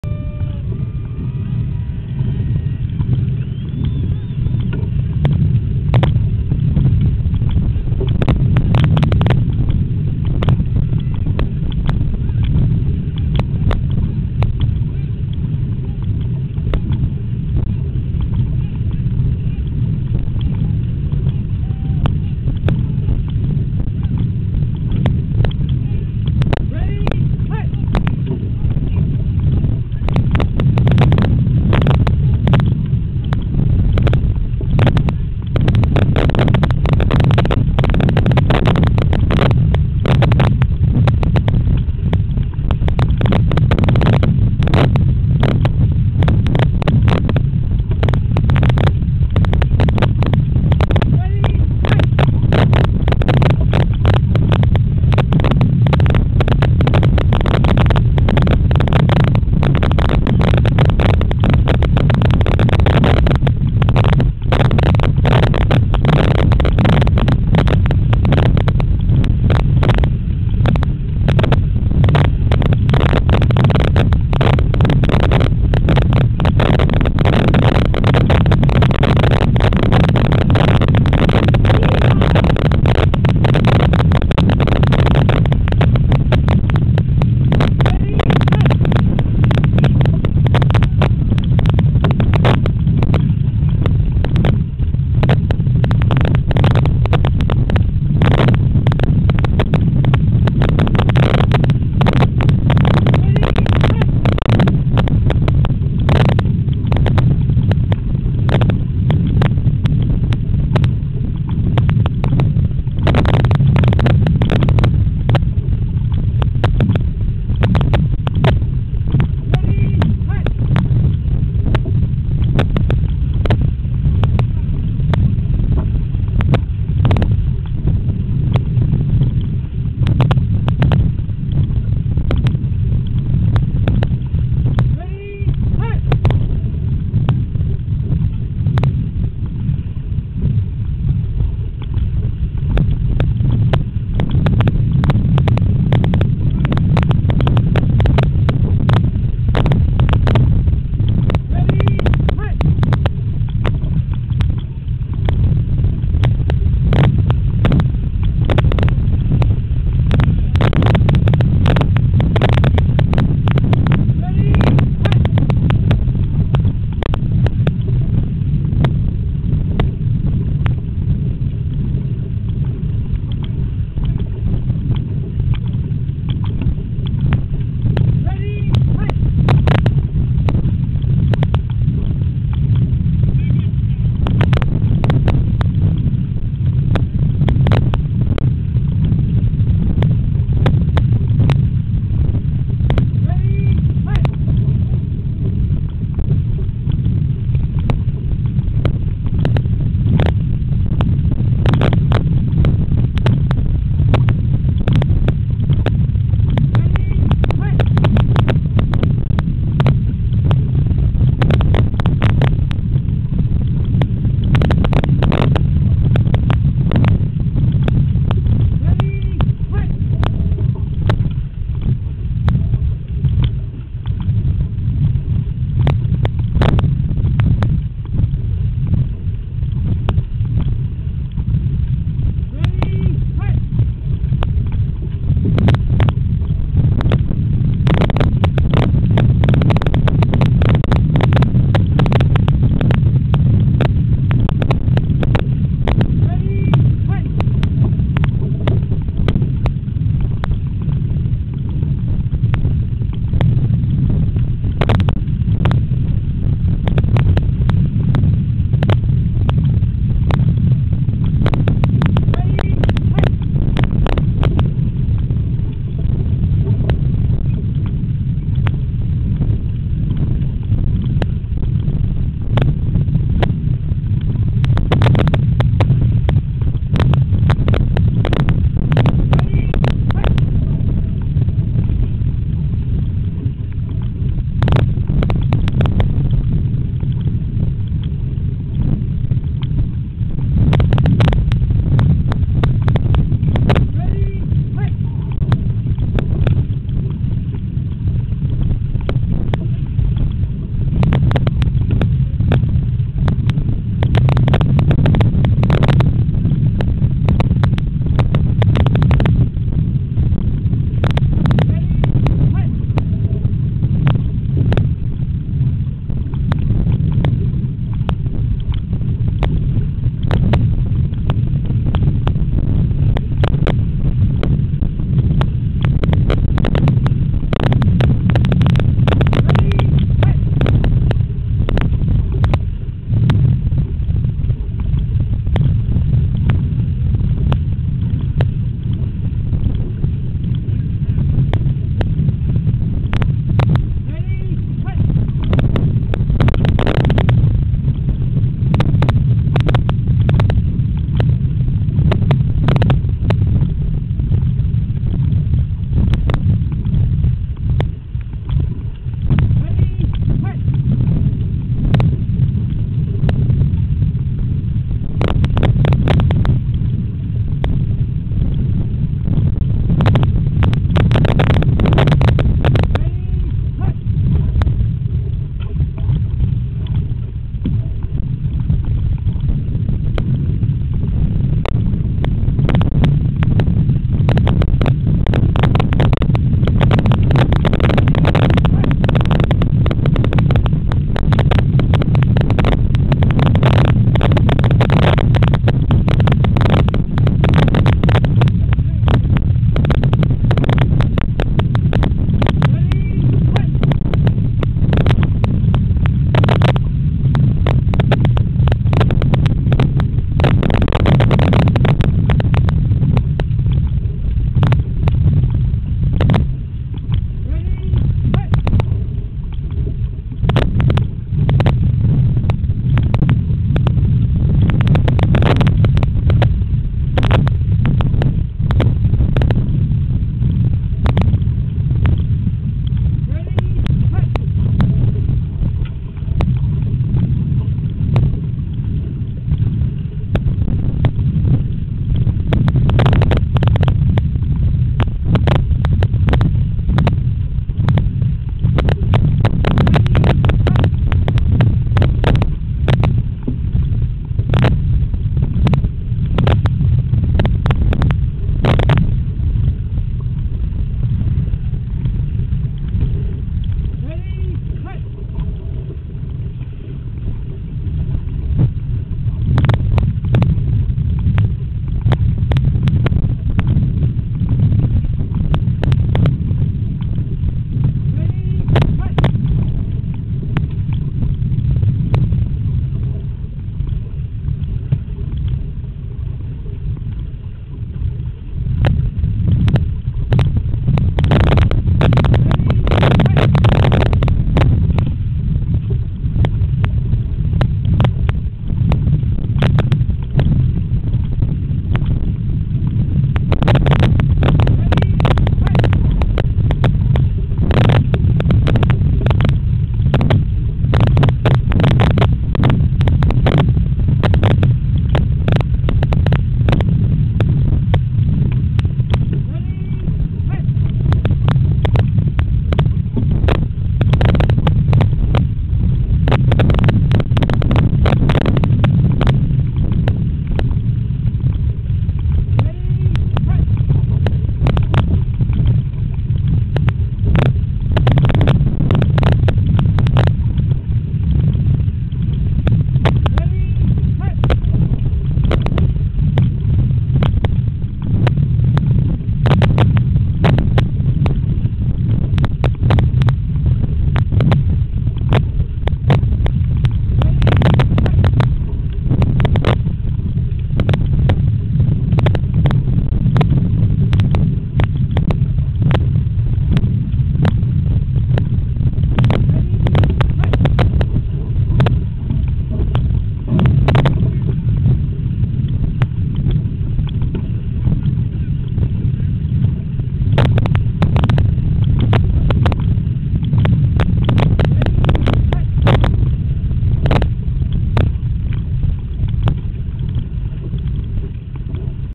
90 Miler 2009 Destroyers Part 1
Dale Krapf's Team of Destroyers!
Dale Krapf, Todd Roadman, Erik Werner, Kevin McFadden, Tom Yarosh, Dell Cummings, Jeff Rankinen